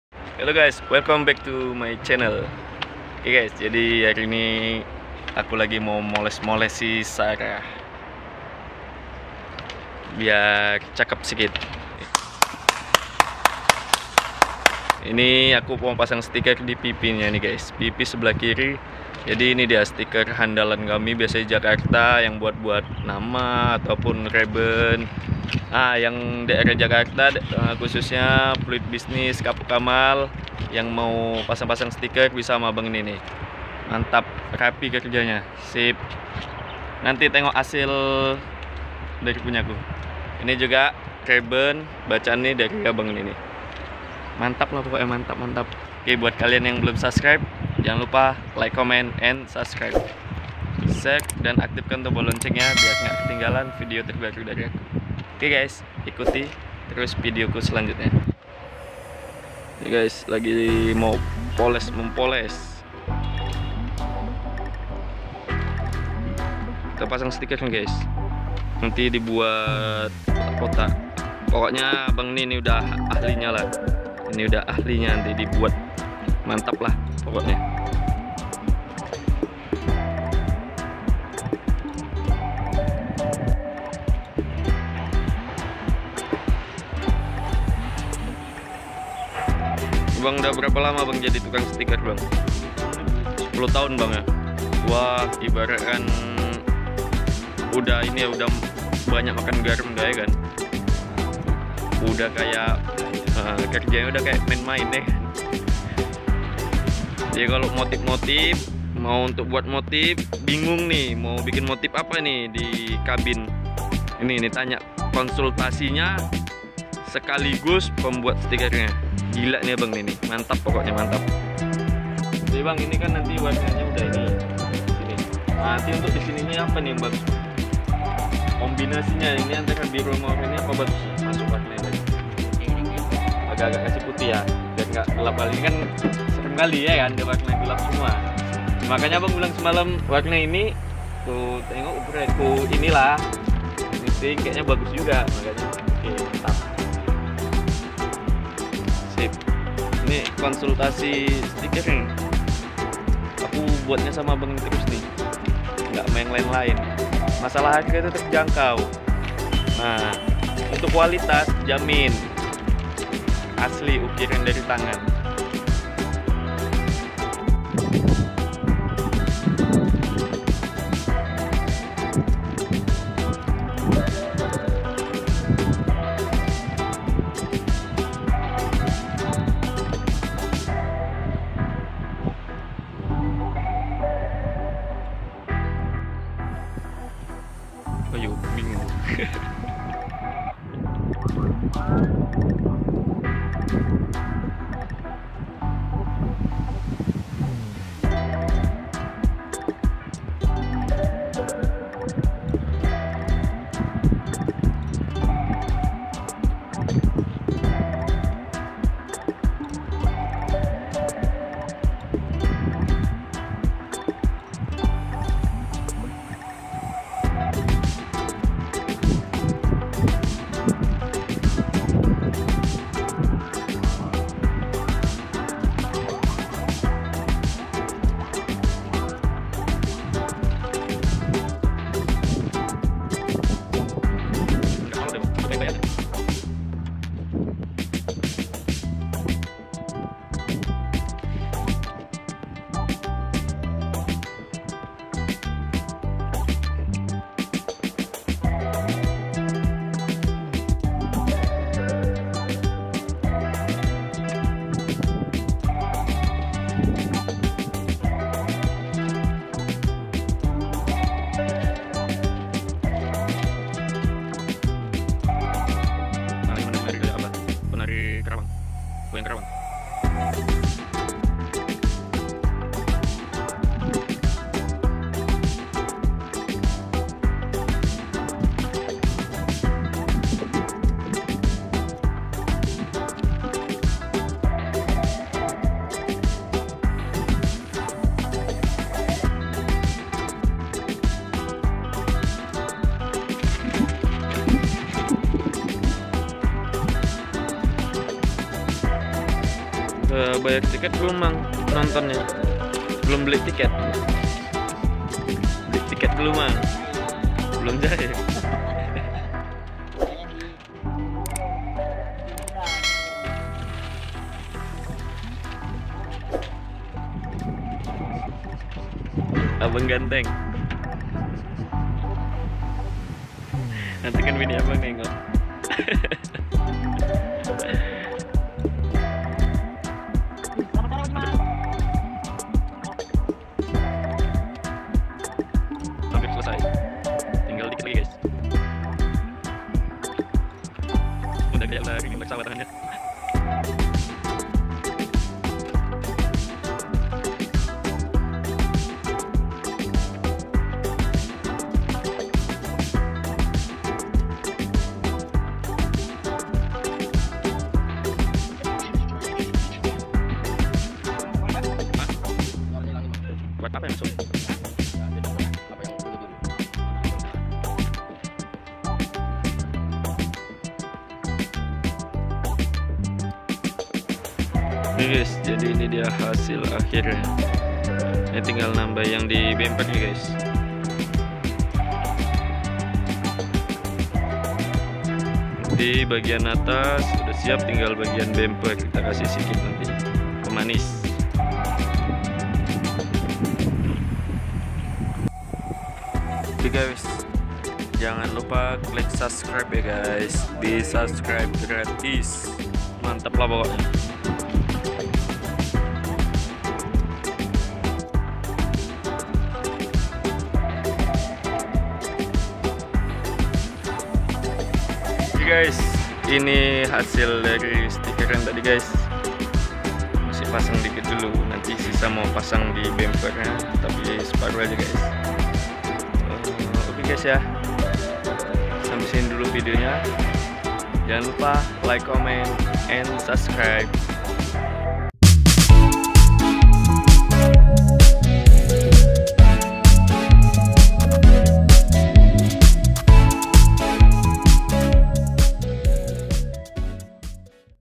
Truk trinton Fuso super great pasang stiker kombinasi di kabin sebelah kiri//truk lintas
Biar makin cakep,pasang stiker kombinasi 2 warna di truk trinton Syarah truk panjang Sumatera,setelah ini mau brangkat muat dan berangkat ke Sumatera

truk panjang siap meluncur